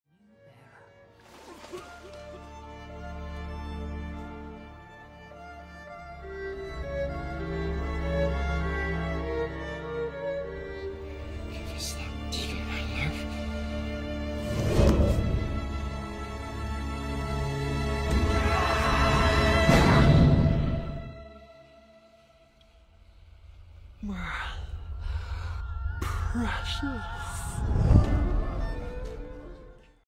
My Precious - A Drawing of Gollum
As requested by my IG followers.. A Drawing of Gollum. Had so much fun filming and drawing this piece!! Hope y’all enjoy 💖💖